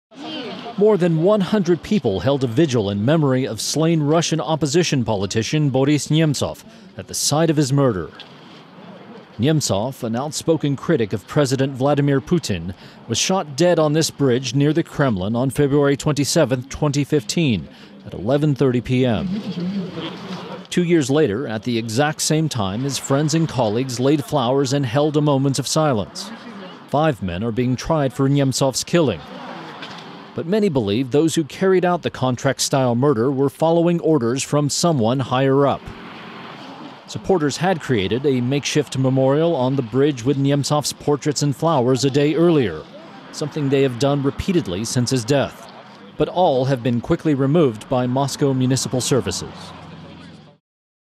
Russians Mourn Putin Critic Nemtsov On Murder Anniversary
More than 100 people held a vigil in memory of slain Russian opposition politician Boris Nemtsov at the site of his murder on February 27, 2015. (RFE/RL's Russian Service)